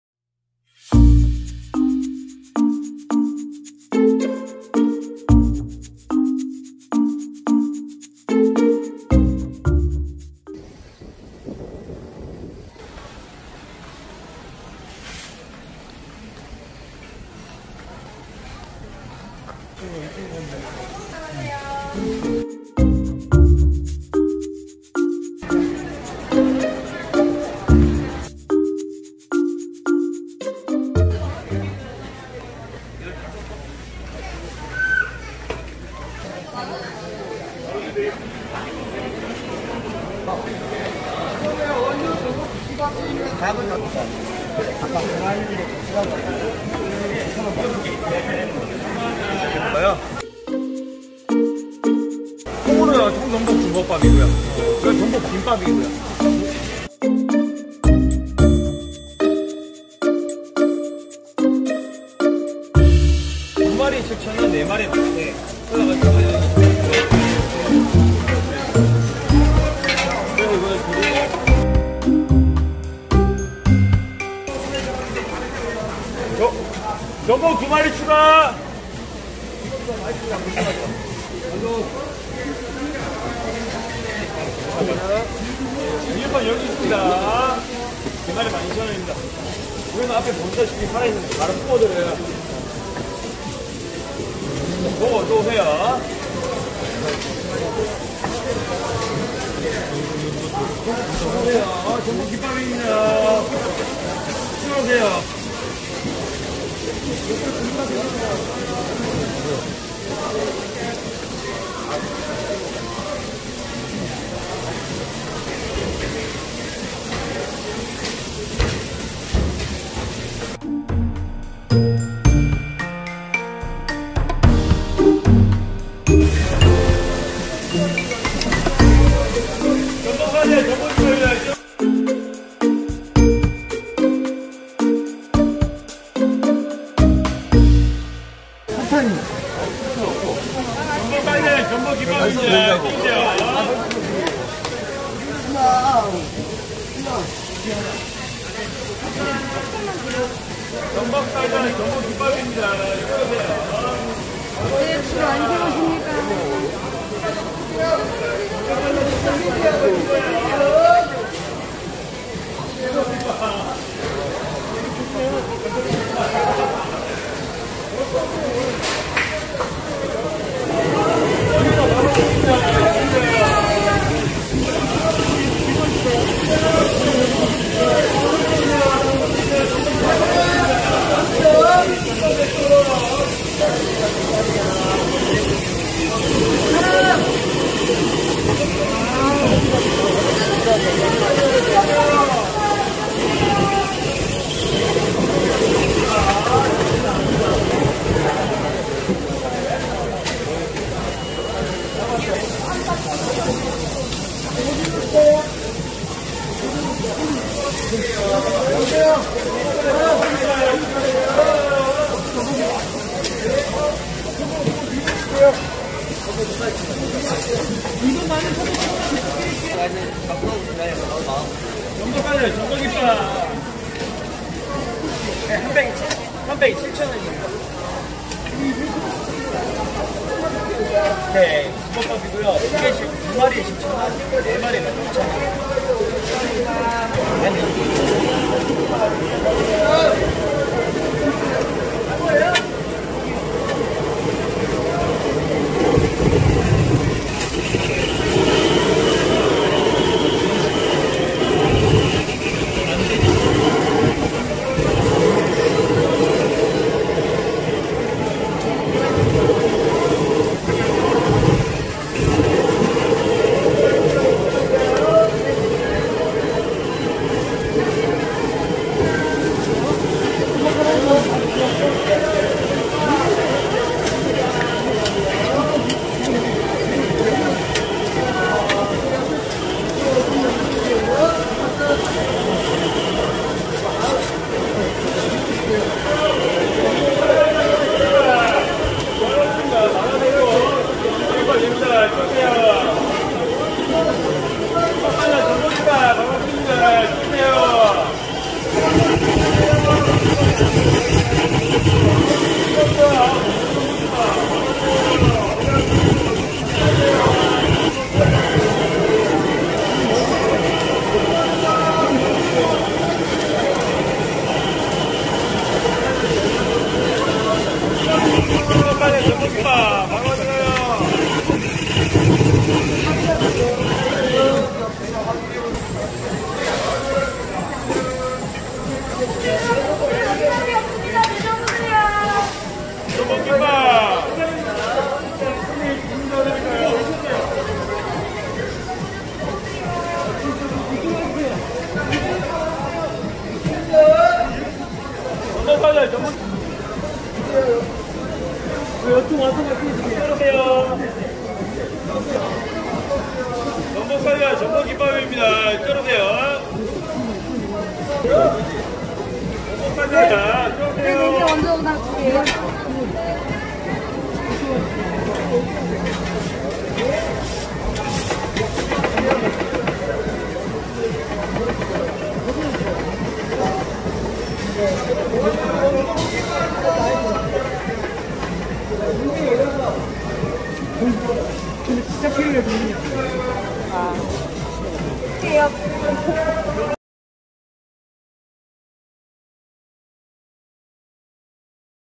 【韓国チェジュ島】 市場ののり巻き 제주여행 동문시장 전복김밥 Korean street food
韓国の済州島にある市場で有名な食べ物、のり巻きです。そこに行くと食べないといけないものです。

It's Abalone Gimbap in Dongmun market in Jeju island, Korea. You should try when you have a chance to visit there.

Google map
Dongmun Market (제주 동문시장)